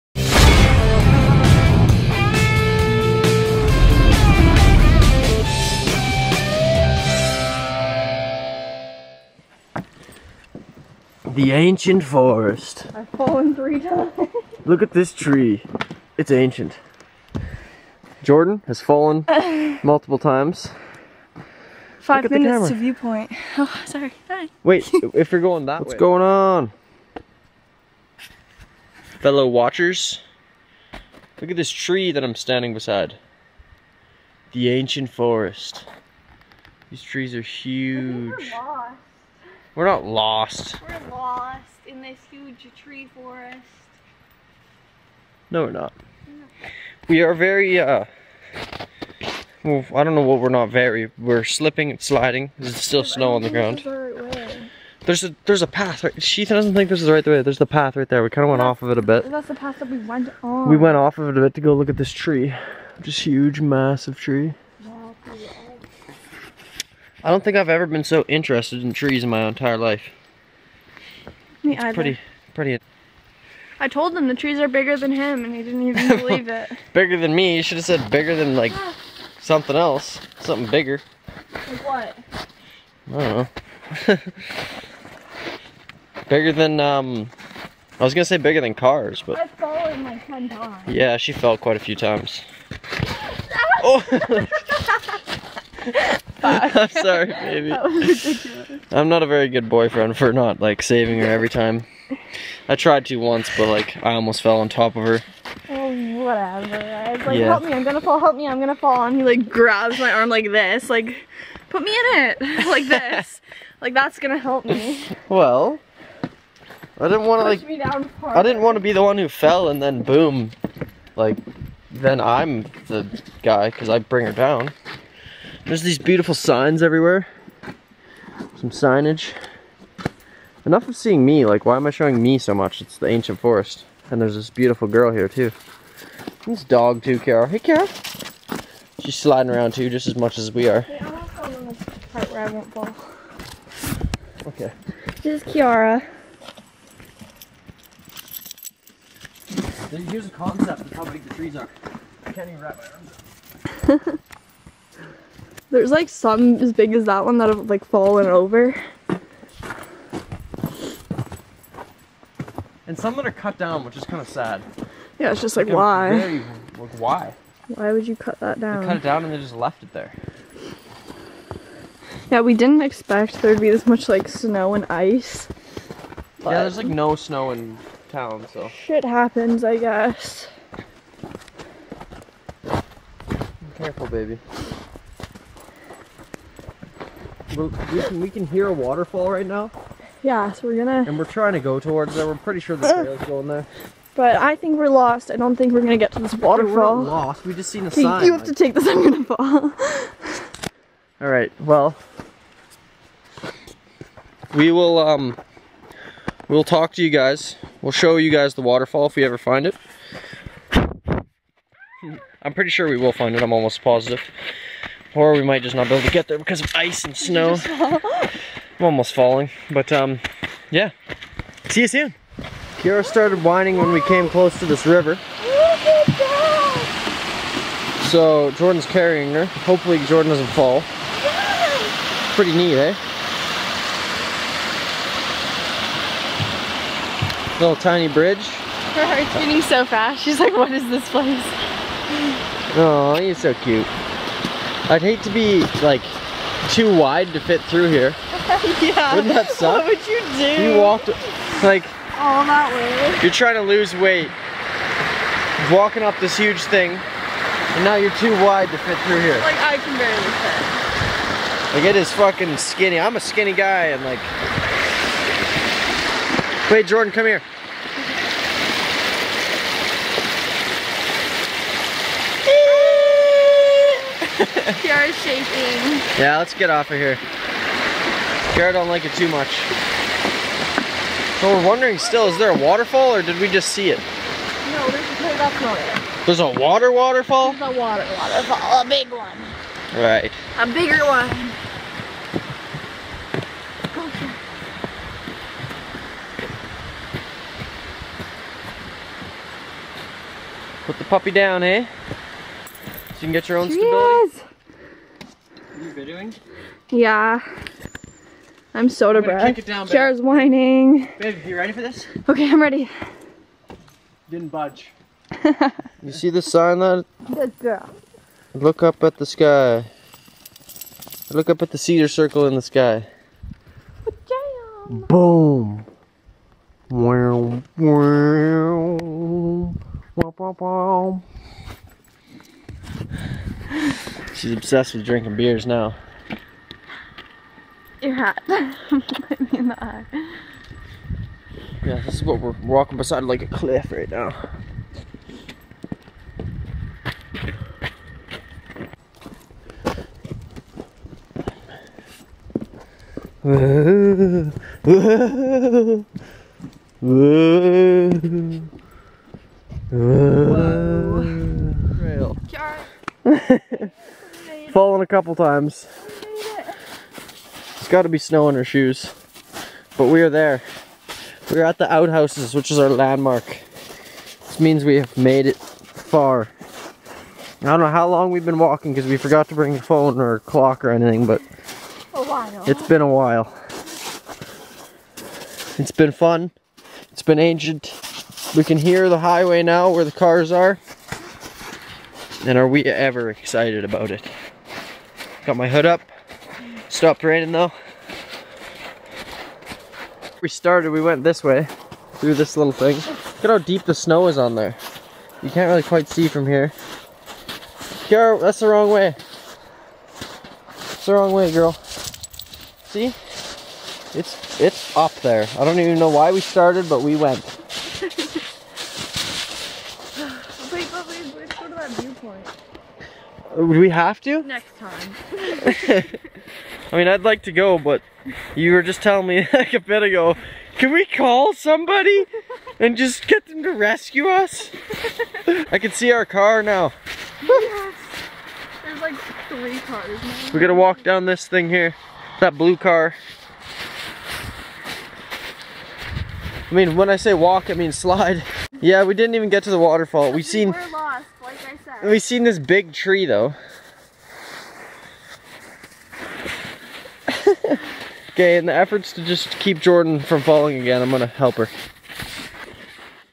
VLOG | HIKING IN THE ANCIENT FOREST GONE WRONG
It seemed to look like a nice day for a hike but once we got to the forest it was full of snow and ice! We enjoyed the beautiful view and the different kinds of trees , however Jordan was mostly focused on keeping her balance (pregnancy clumsiness) .Thanks for watching and subscribe for more videos coming soon.
Jordan
ask.fm - jbabss